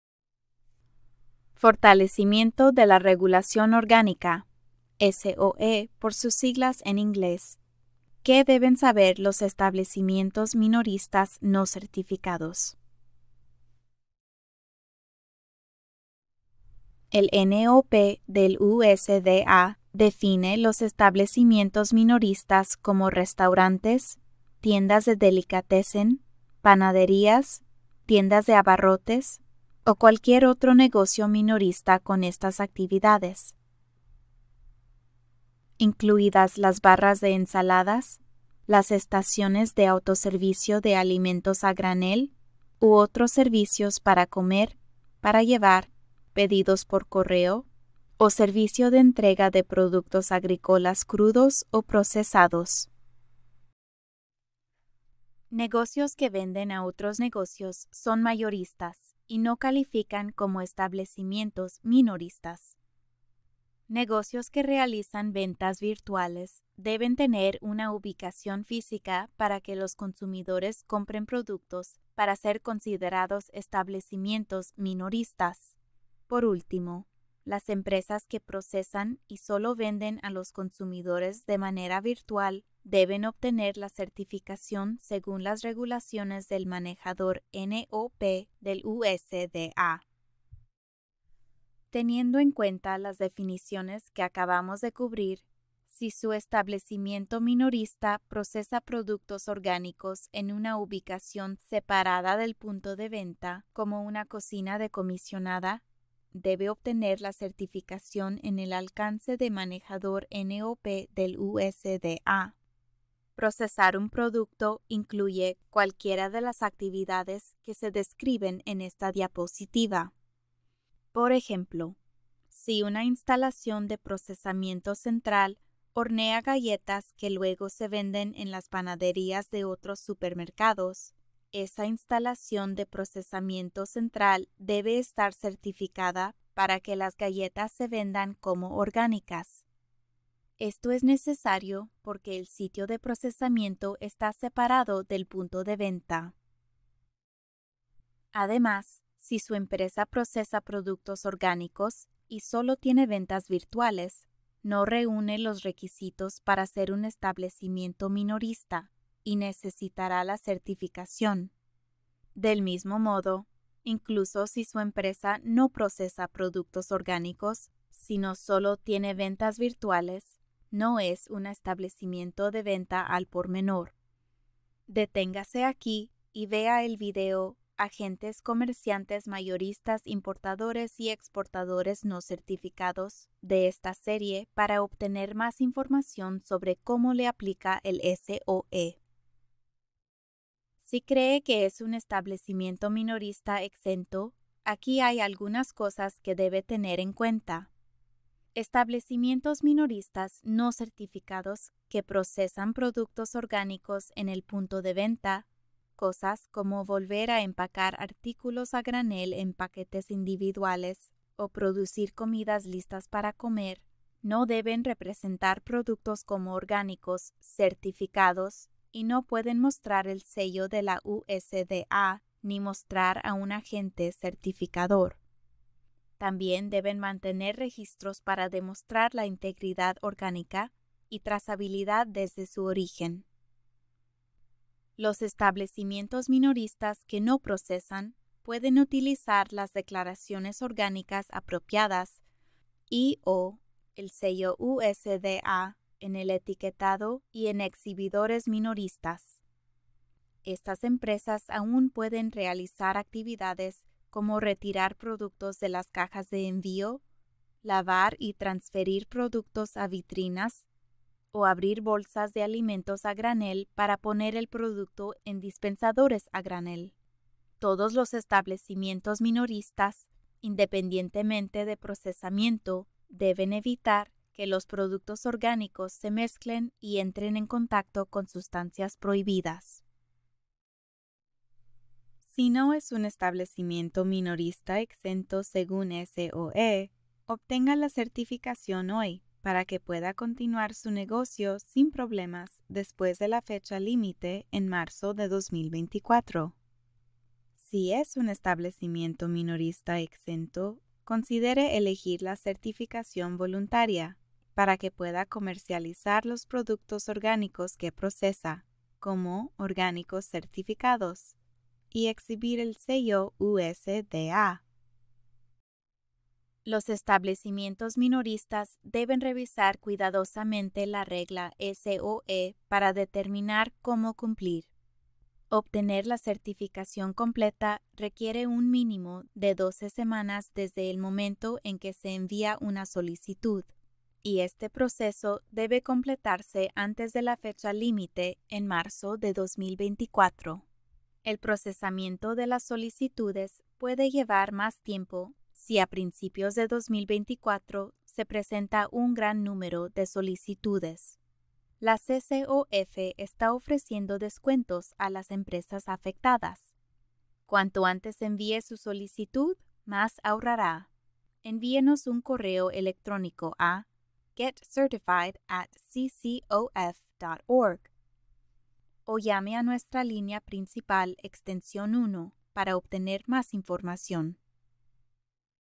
SOE: Lo que necesitan saber los establecimientos minoristas no certificados
El Fortalecimiento de la Regulación Orgánica (SOE) es el cambio más significativo a las regulaciones orgánicas nacionales desde la creación del Programa Orgánico Nacional del USDA. La actualización pretende mantener la confianza en los productos orgánicos y minimizar el fraude a lo largo de la cadena de suministro orgánico. Todas las operaciones -incluidos los manejadores de productos orgánicos actualmente certificados y los nuevos manejadores de productos orgánicos no certificados- deben cumplir con todos los requisitos de la norma antes del 19 de marzo de 2024.

Vea nuestros vídeos de esta serie para conocer más sobre cómo puede afectarle el SOE y qué puede hacer para cumplir los requisitos.